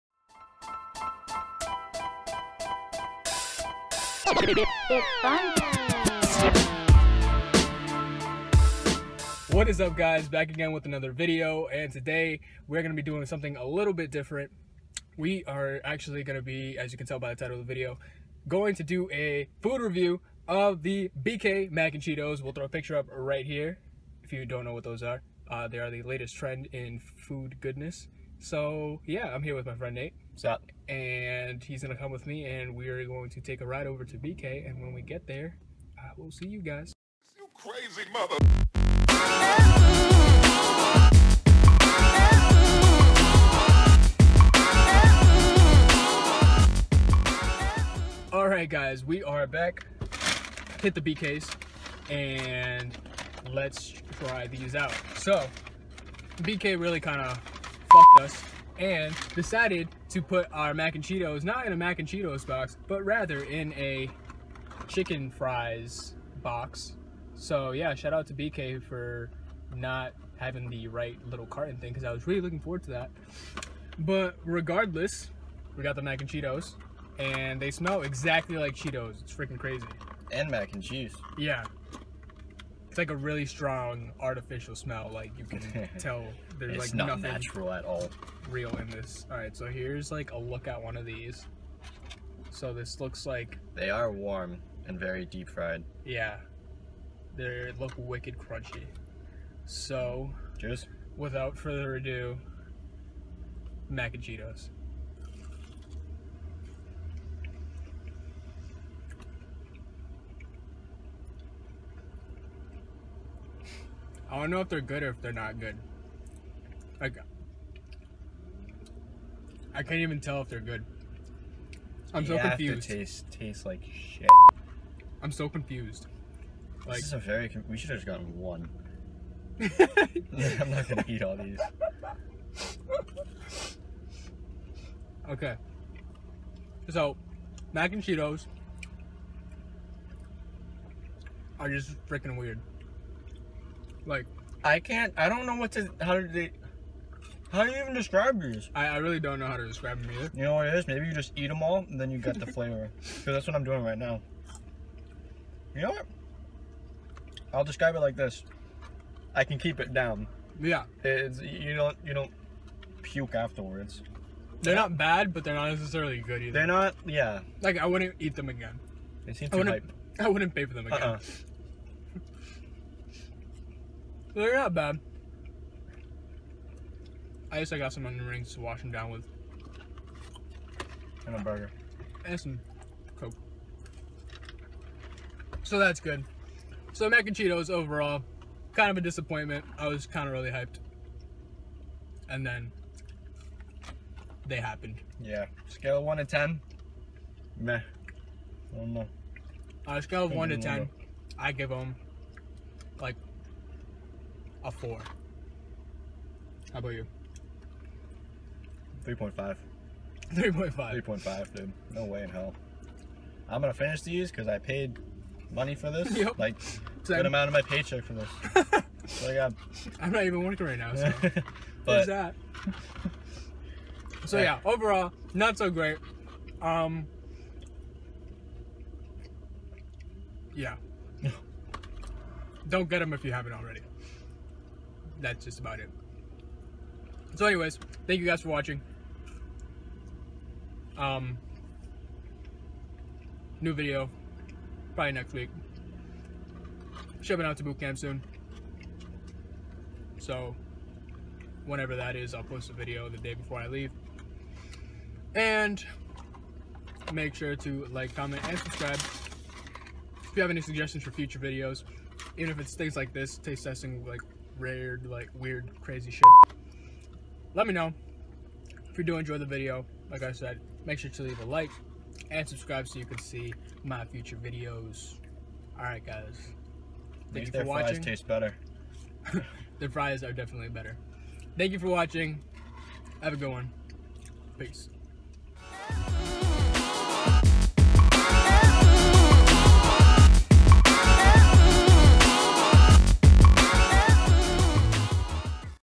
MAC N' CHEETOS REVIEW
Today my friend Nate and I decided to head over to Burger King to try their BK Mac N' Cheetos. This is our review.

***I couldn't get the Mac N' Cheetos picture to overlay, sorry.***

MAKE SURE TO SHARE THE VIDEO

If you have questions or video requests make sure to leave them down below in the comments!